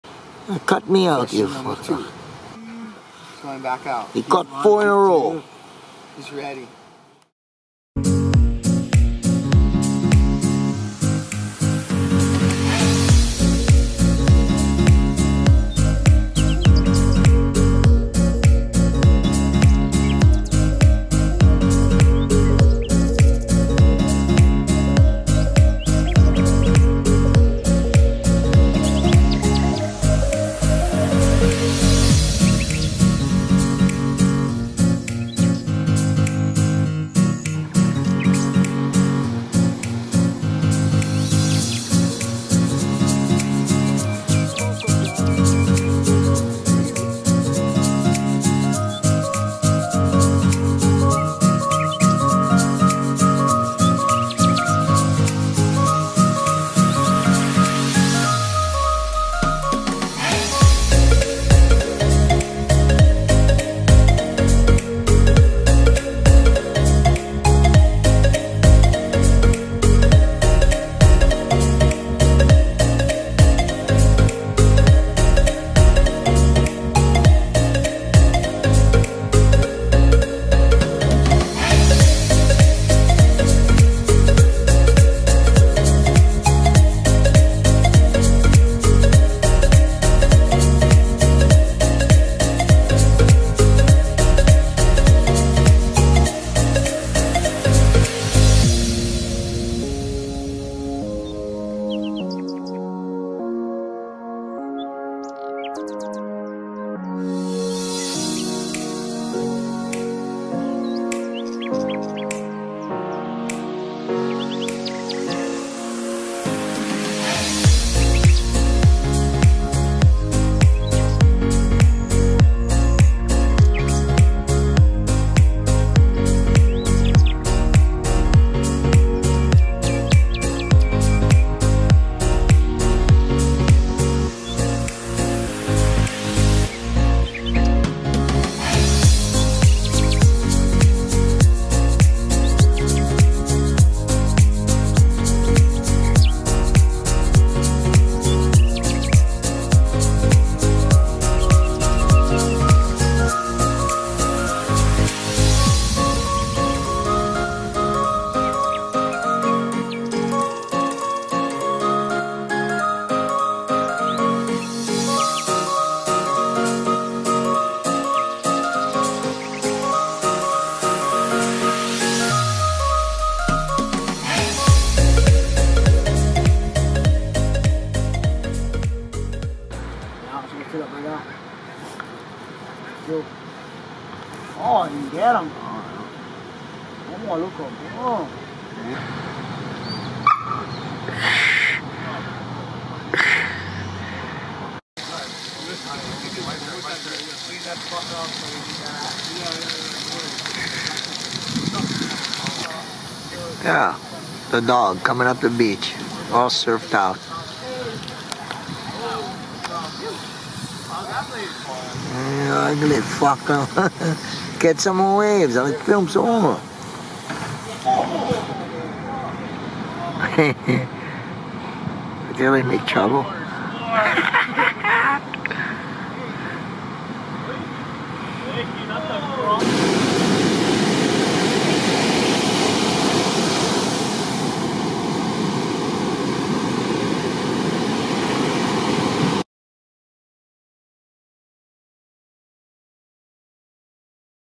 FIRST WINTER SWELLS AT HOME
Sheldon surfs his home break with friends